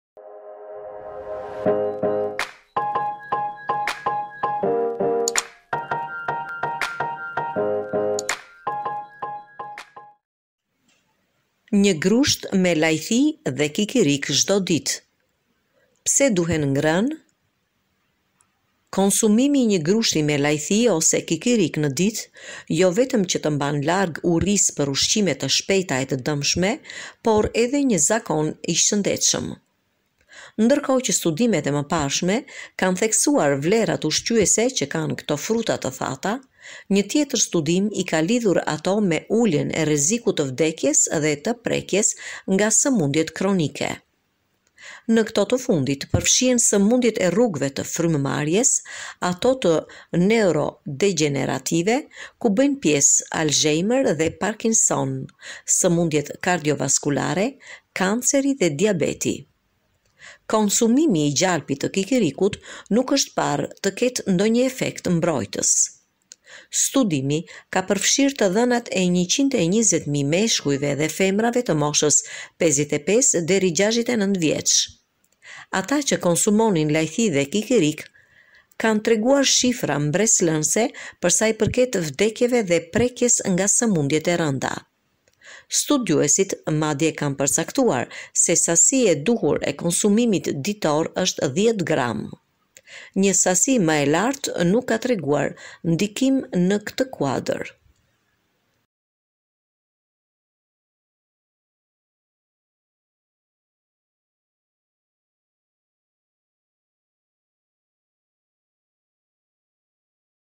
Një grusht me lajthi dhe kikirikë çdo ditë: Pse duhen ngrënë?
Konsumimi i një grushti me lajthi ose kikirikë në ditë, jo vetëm që të mban larg urisë për ushqime të shpejta e të dëmshme, por edhe një zakon i shëndetshëm